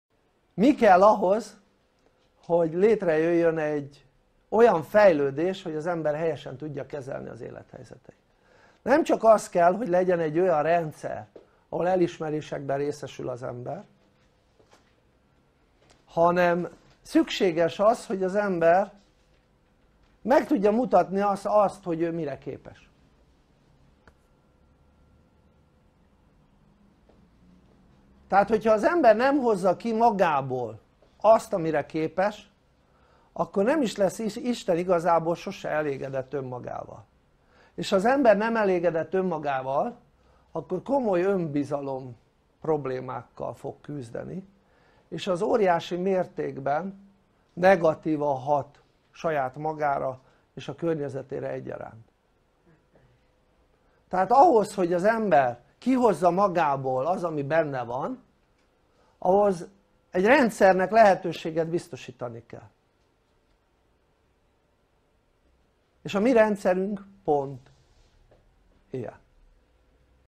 Mi kell az igazi elégedettséghez? - Nyílt Akadémia tudástár
NYÍLT AKADÉMIA tudástár
Előadó: Szedlacsik Miklós mester-coach, boldogság-specialista, a Nyílt Akadémia alapítója
A tudatosság és a képességek fejlesztése az egyetlen út a boldog élethez.
Tegyél Te is magadért és másokért, a boldog életért!